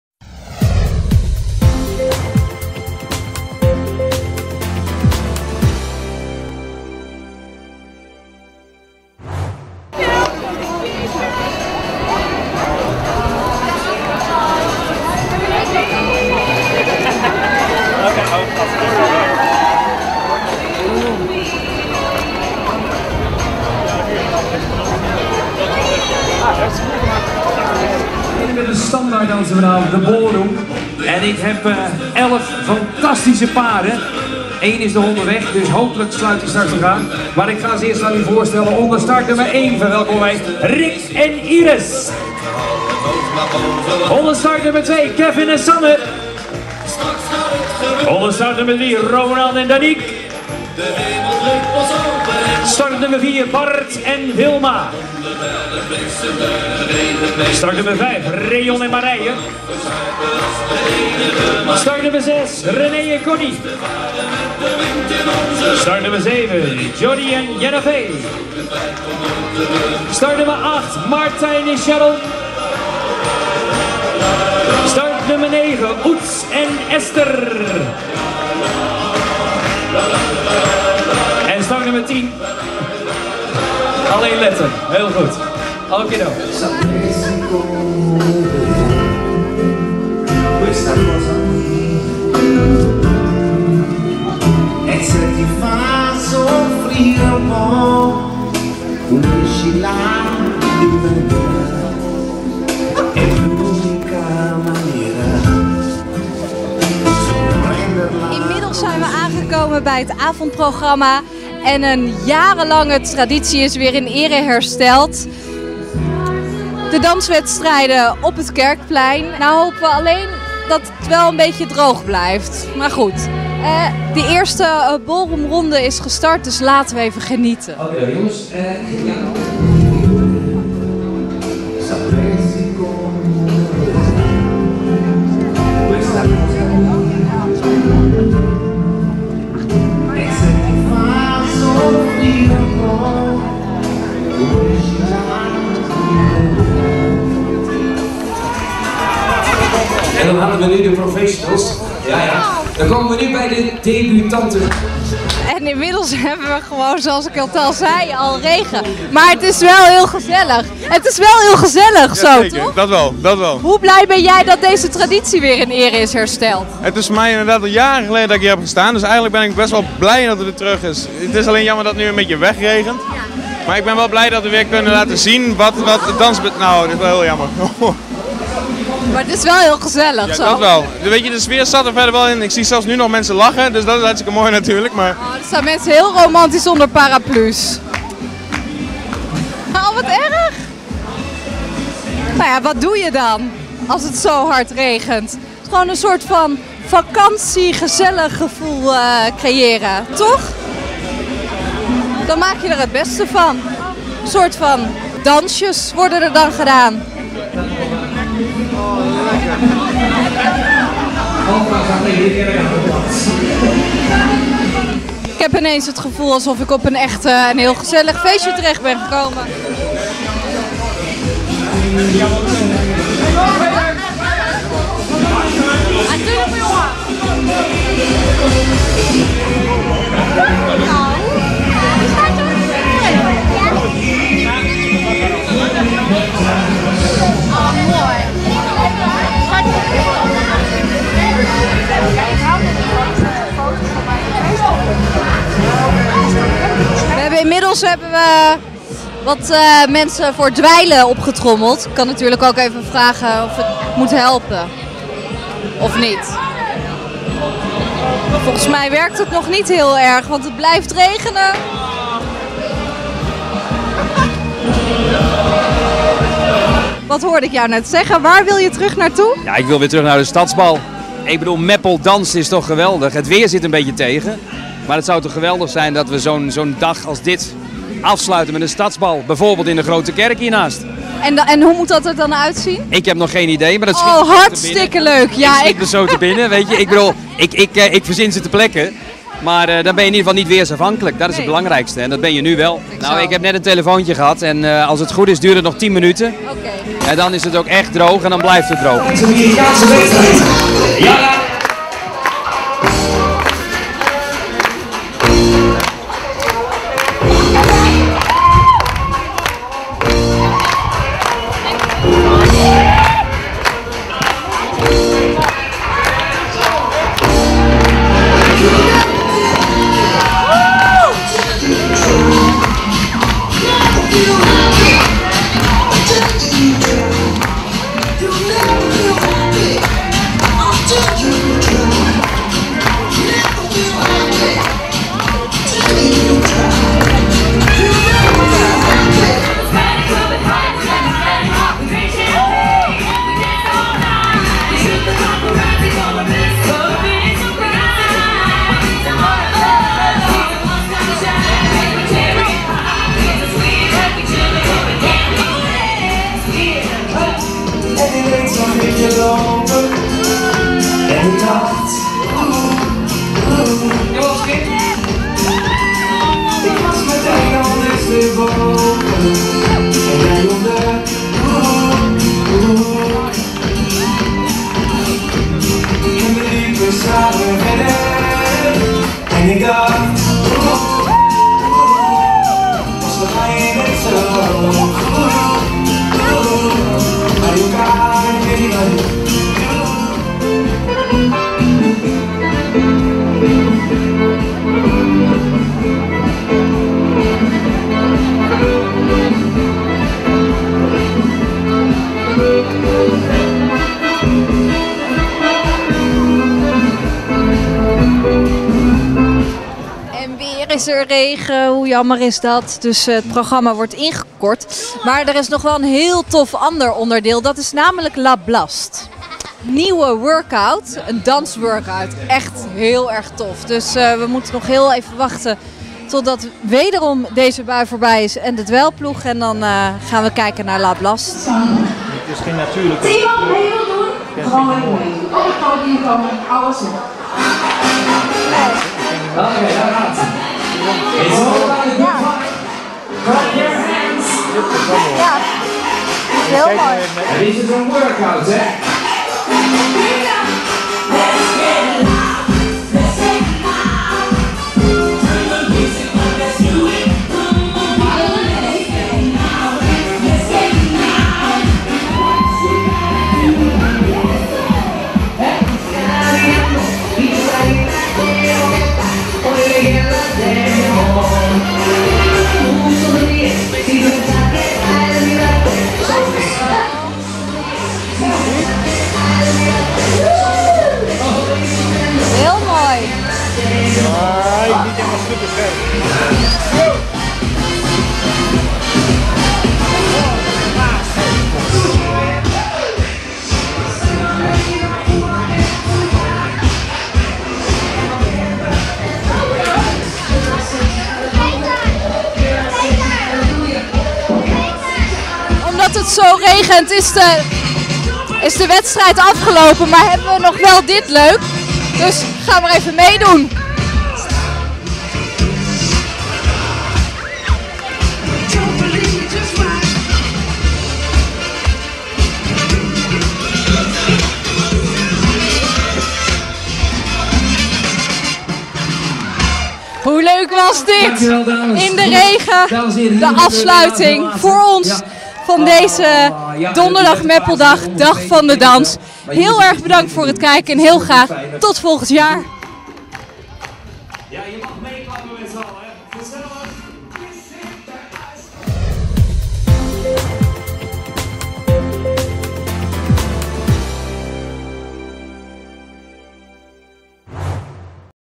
DMD 1: Meppel danst in de regen
Het avondprogramma bestond uit een danswedstrijd en diverse demonstratiedansen. Het geheel viel helaas een beetje in het water, door enkele hoosbuien. De stemming was er niet minder om.